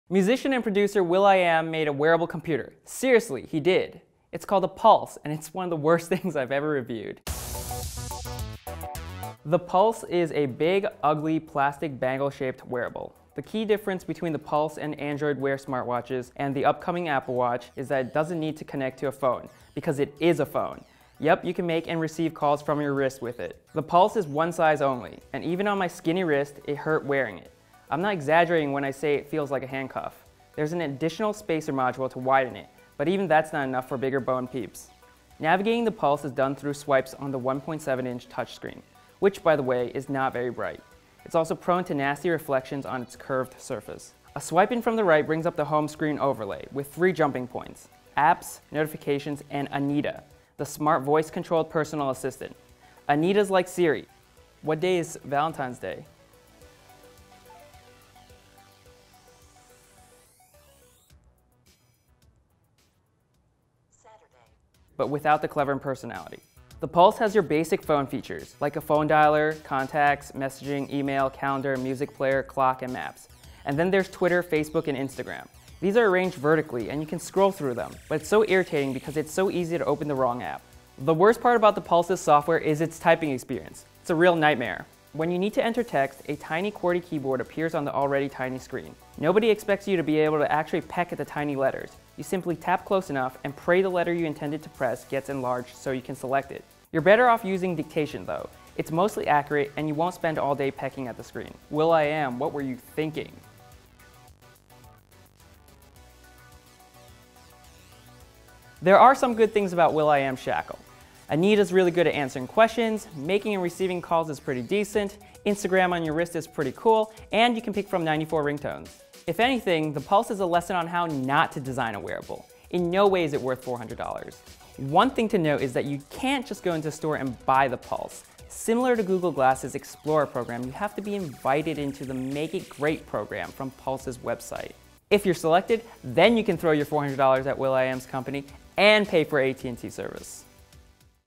Review: Will.i.am's Puls | Mashable
Uncomfortable, chunky and harsh-on-the-eyes, musician Will.i.am's new Puls smartwatch is a lesson in how not to make a wearable.